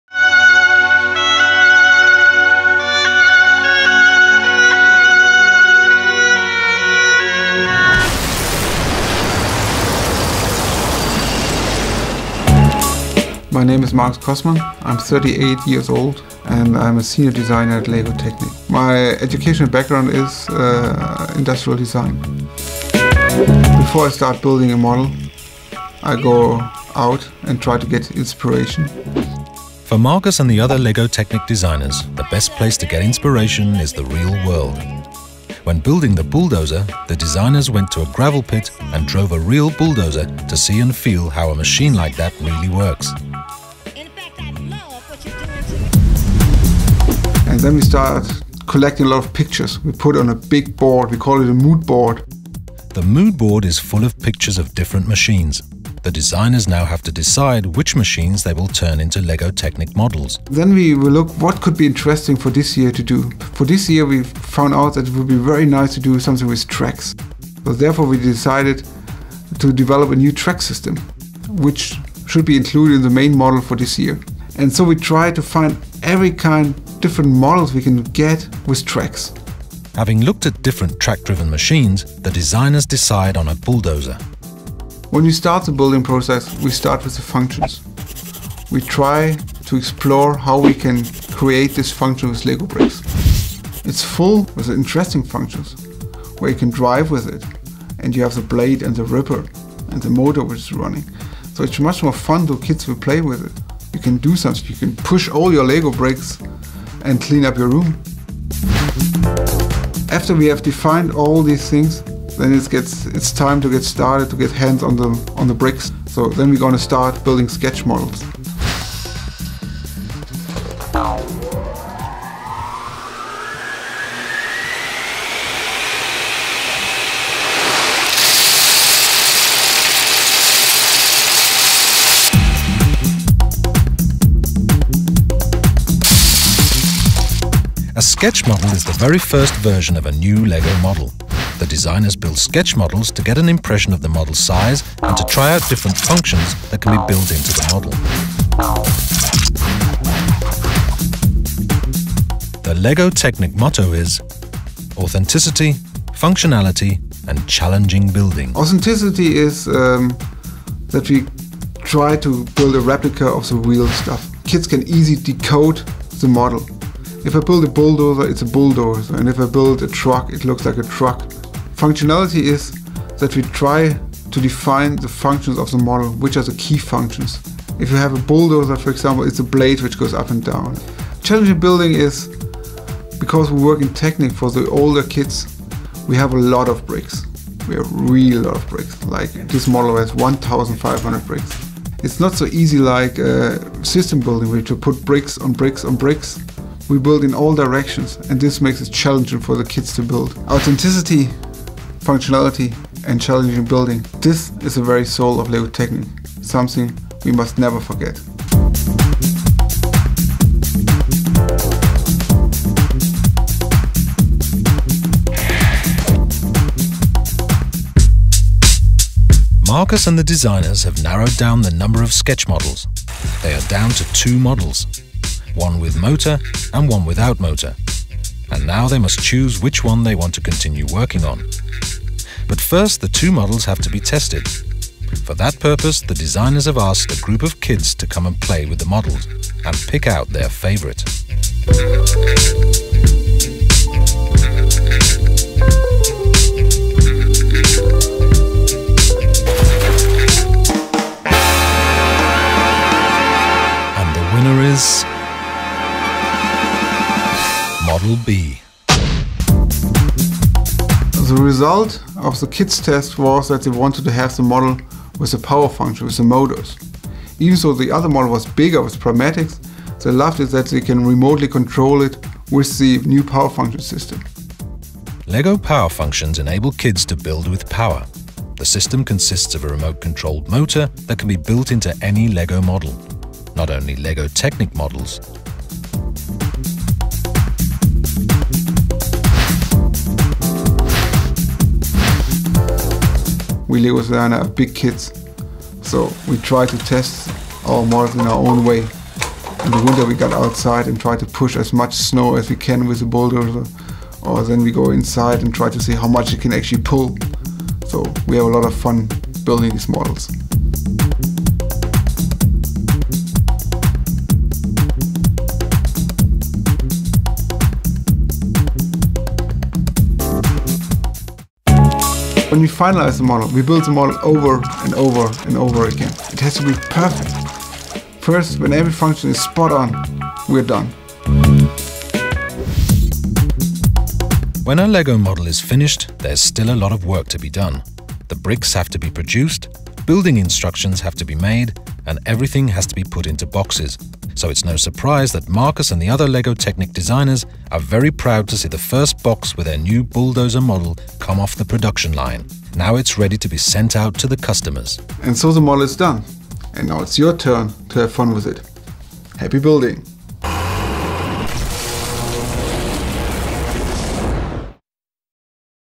LEGO TECHNIC, Behind the Design
A documentary showing how the LEGO TECHNIC Bulldozer came to life.